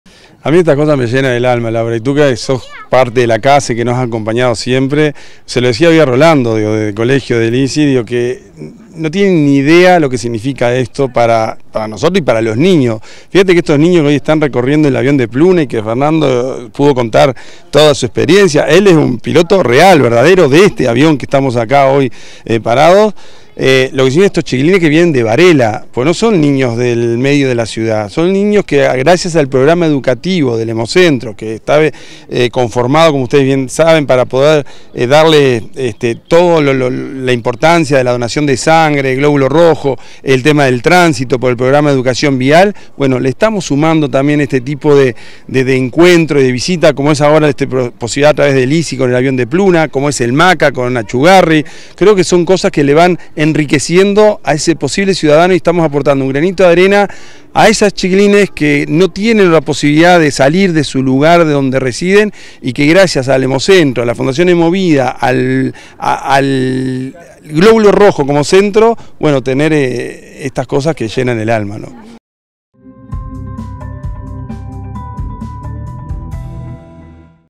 SE FIRMÓ CONVENIO CON EL INTERNATIONAL COLLEGE | Dr. Jorge Curbelo - Director Técnico Hemocentro.
Dentro del programa educativo de la fundación Hemovida